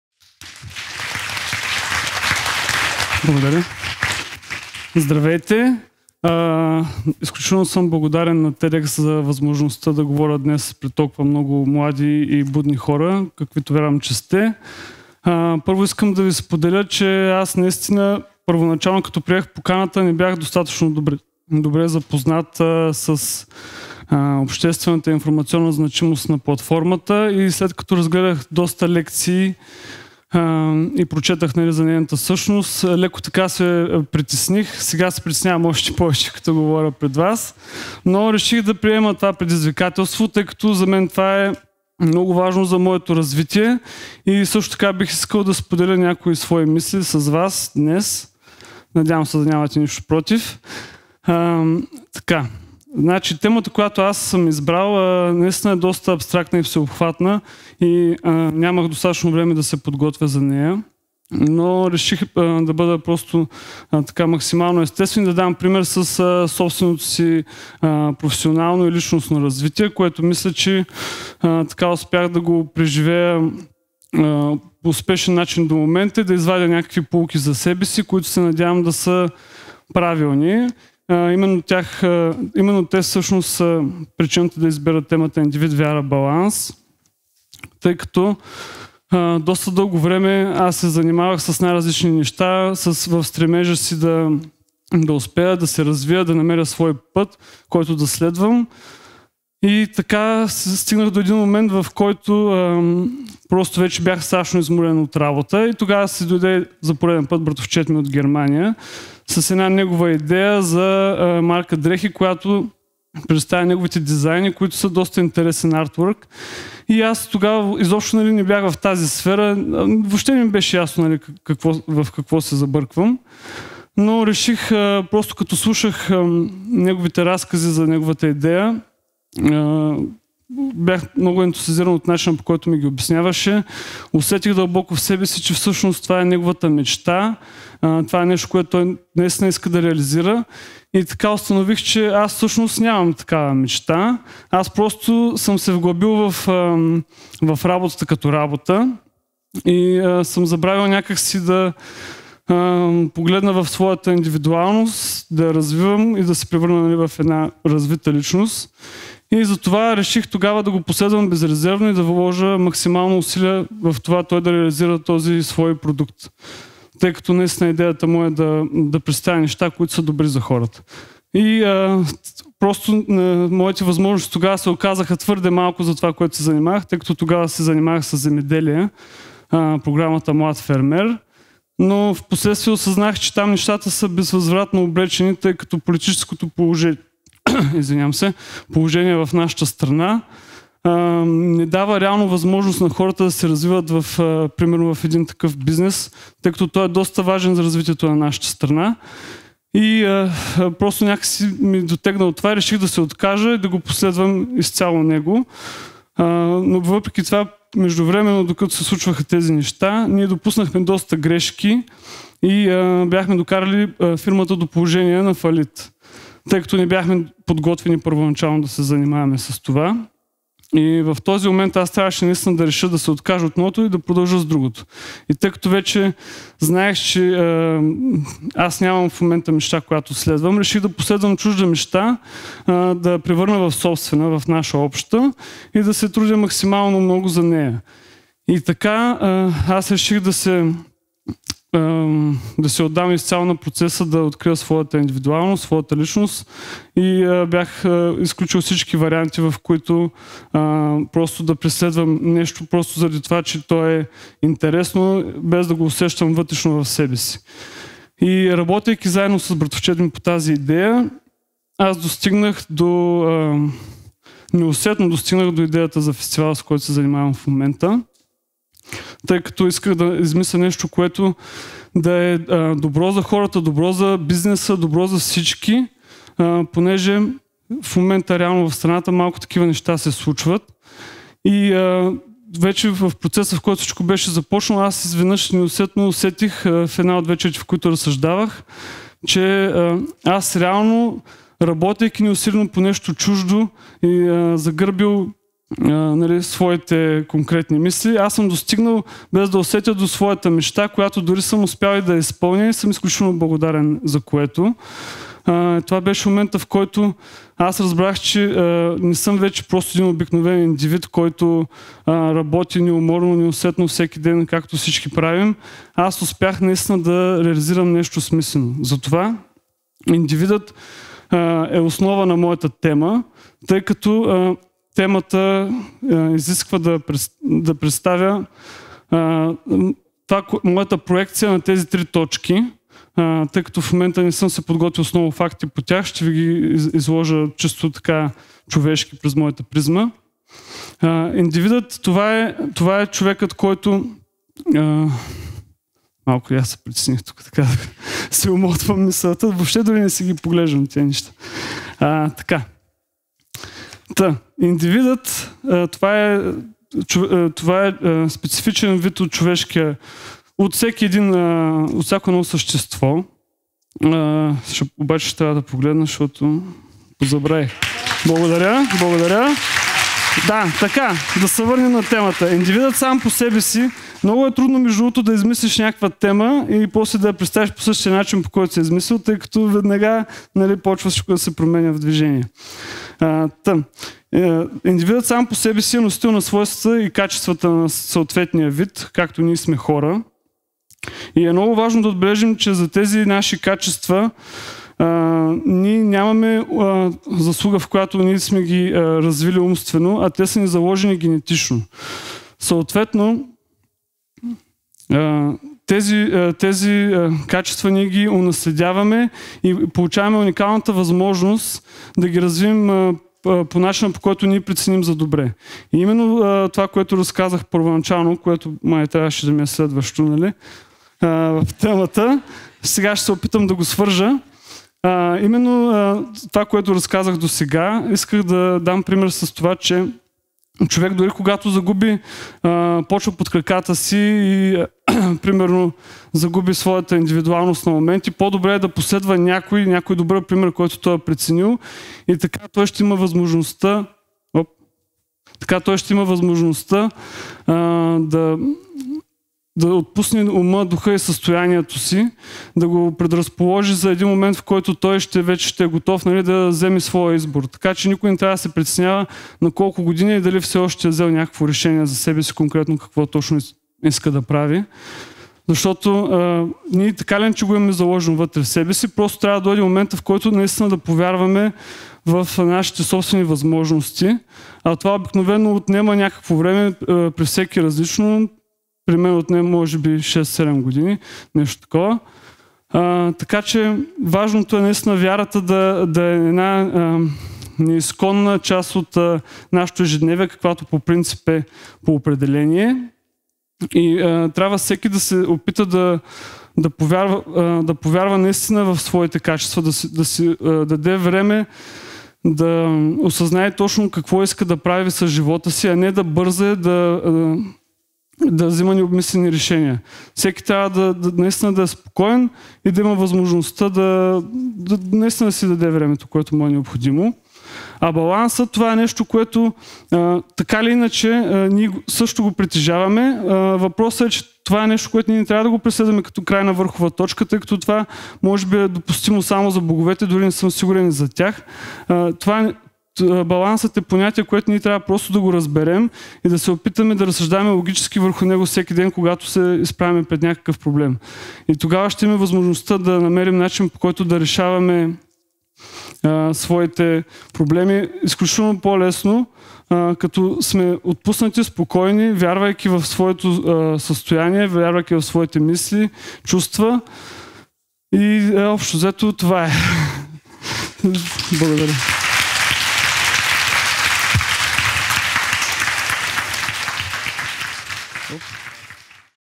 Individual - Faith - Balance | Nikola Sivkov | TEDxStaraZagora
Nikola Sivkov talks about the possible solution of some of the thoughts and issues that everybody has.

Nikola Sivkov is the chief organizer of Kapana Fest in Plovdiv. He is one of the creators of the creative urban brand for street apparel  "What A Monster".